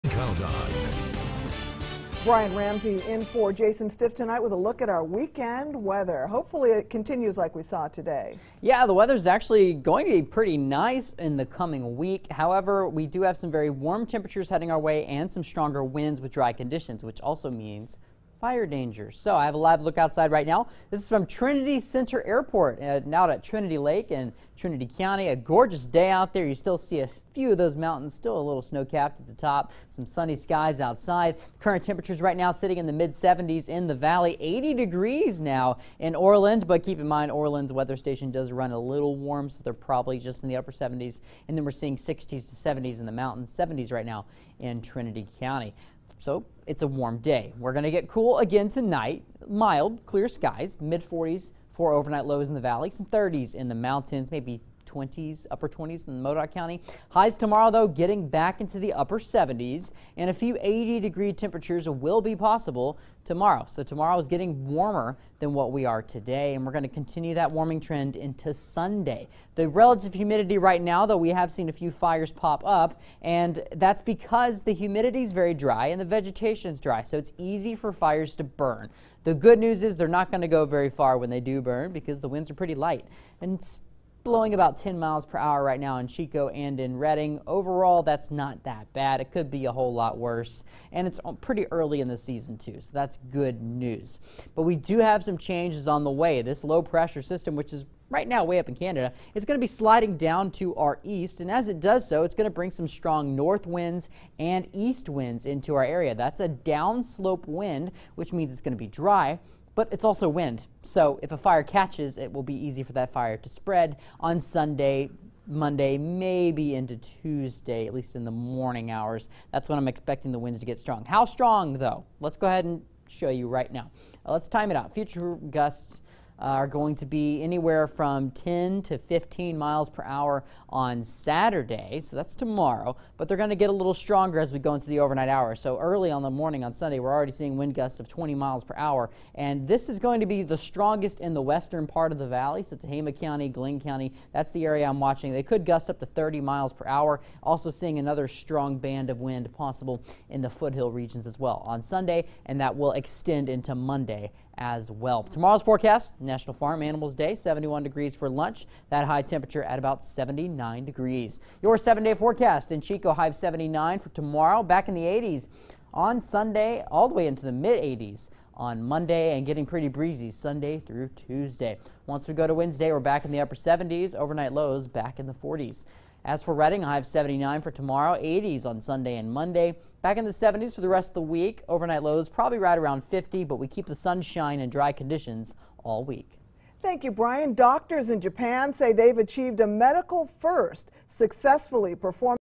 Storm Tracker Forecast: Fire danger returns to NorCal
Dry, sunny and warm weather have lead to small fires popping up over the last week and will continue over the next week. Breezy conditions Sunday and Monday could increase these concerns.